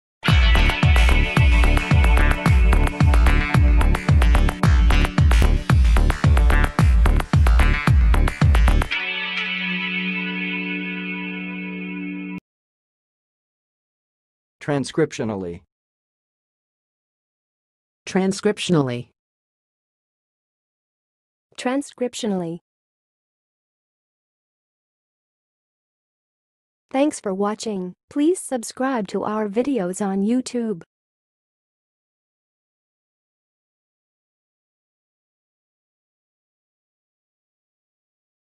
How to say "transcriptionally"! (High Quality Voices)
Watch in this video how to say and pronounce "transcriptionally"!